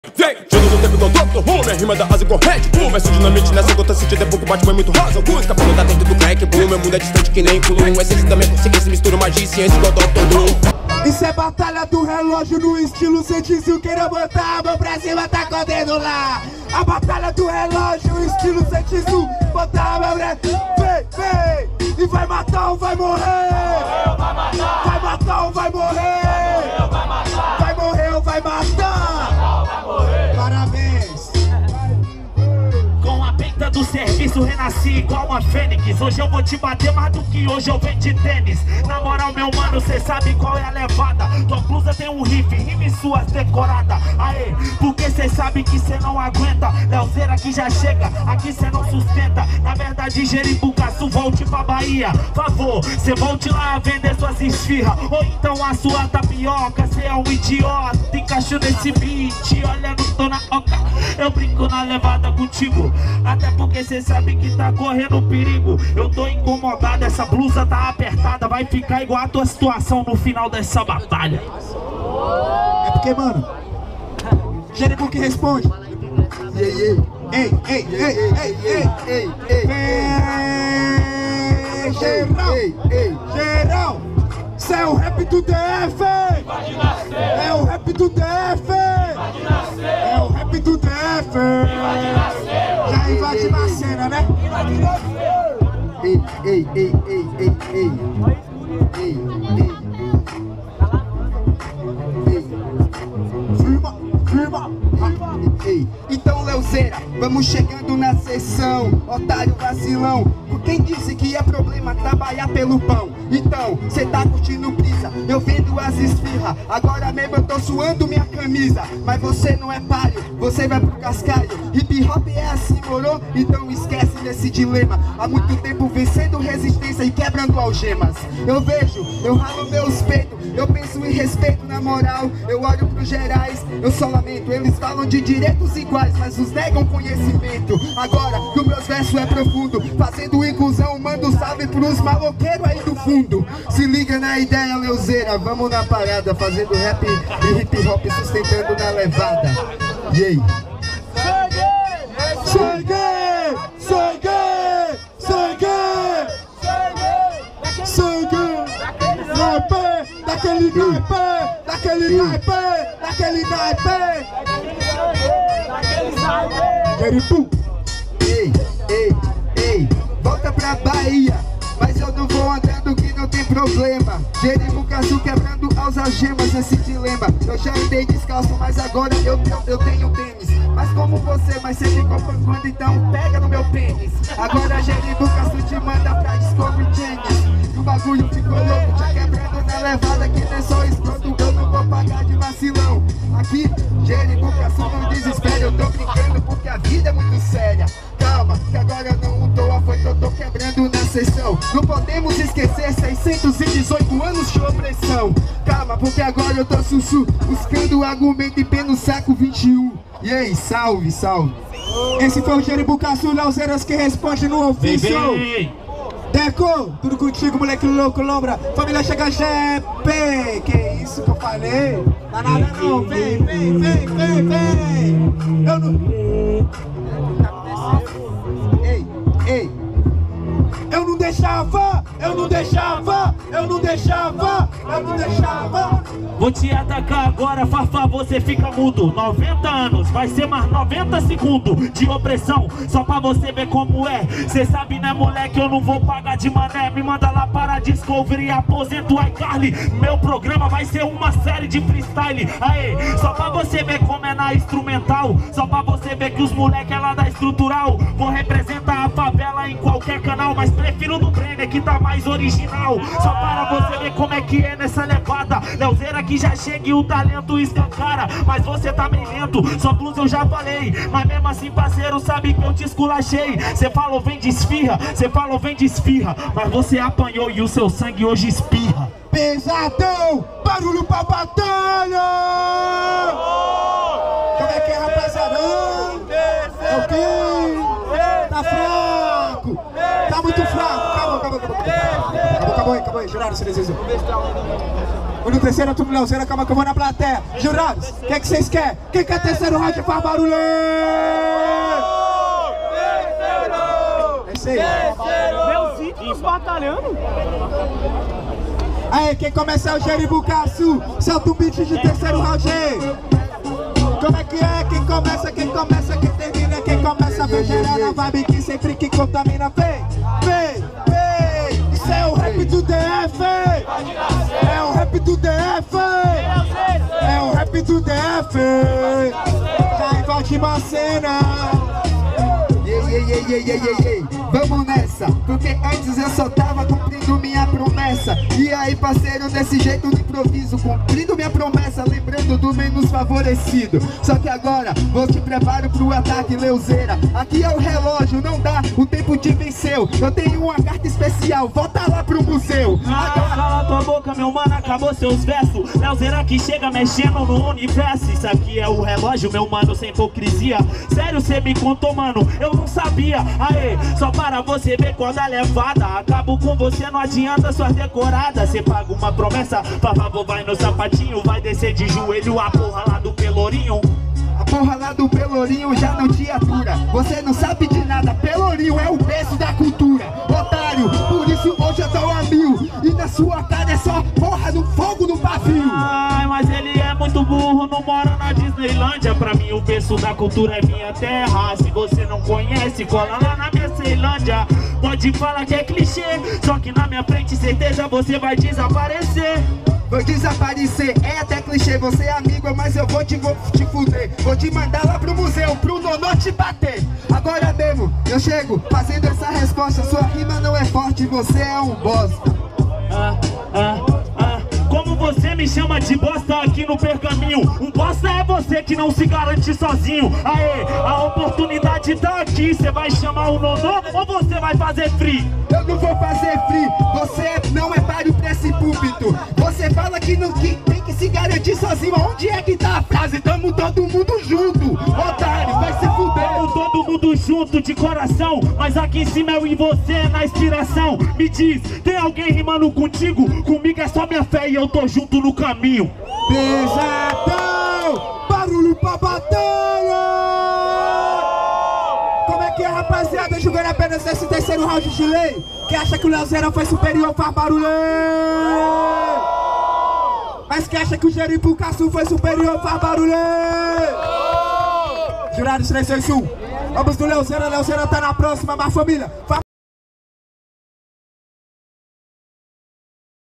Jeribucaçu Vs Leozera - Fase Batalha do Relógio (Taguatinga/DF) - 2018
- INFORMAÇÕES NA DESCRIÇÃO -

Homo Freestyle Sapiens | Nauí MOVNI, Doctor Zumba, Mr. Dreka, Hot & Oreia | Prod. Lp Studio

Apoio:
Black Dog Tattoo Facebook:

➤ Evento: Rap do DF - Batalha do Relógio  - Taguatinga Centro
     Todas as Quintas-feiras às 19:30h